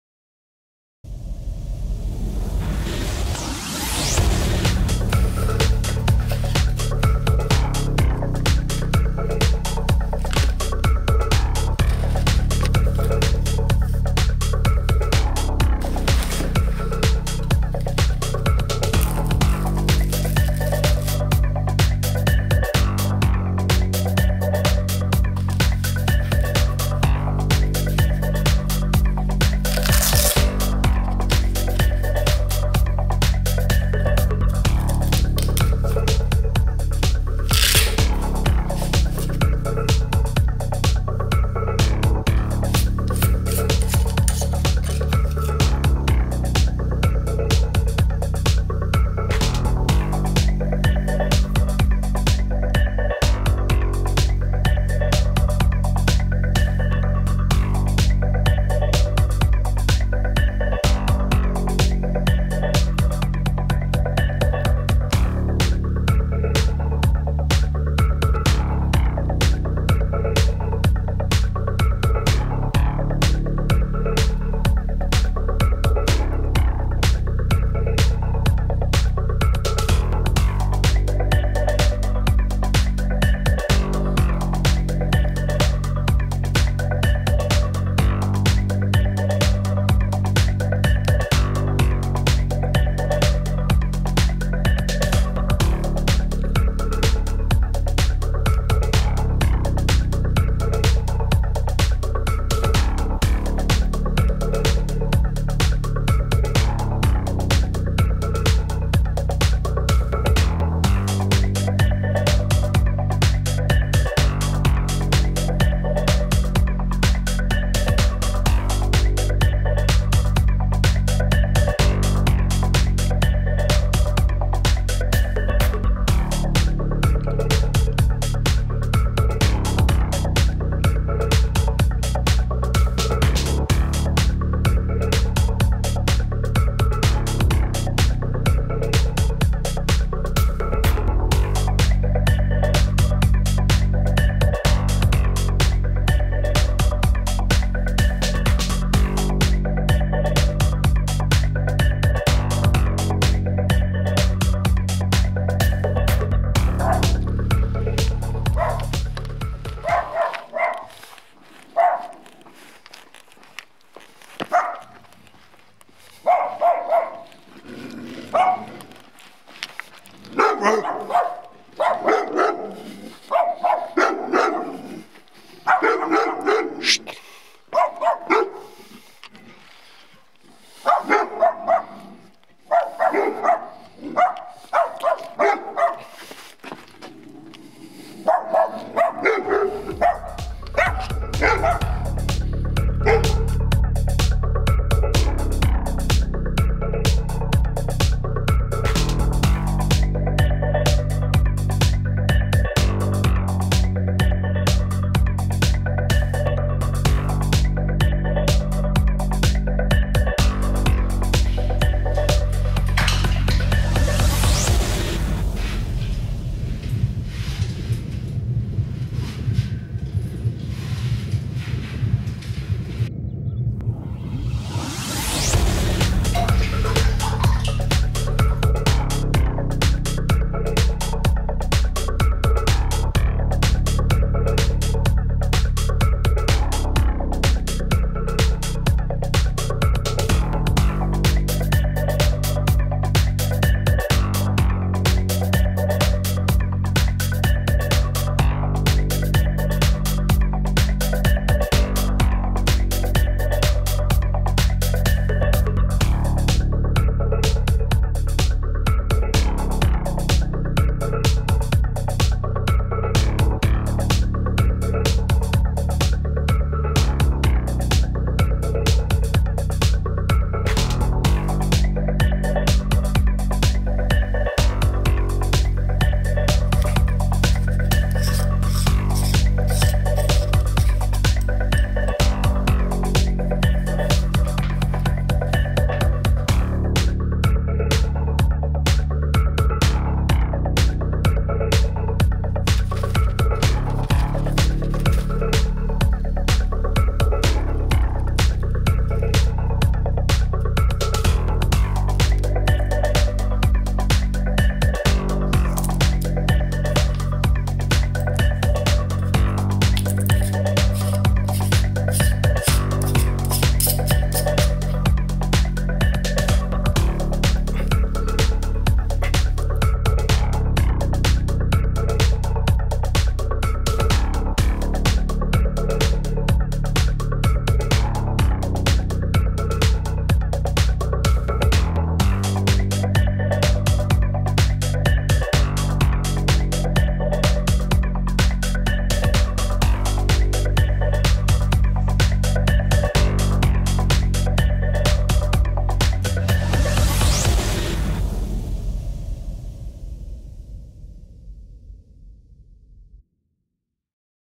Review Nitecore TM06S - Unbox Test Advice
Review of the new TM06S flashlight from Nitecore. The most powerful lamp of this dimension. 4000 lumens.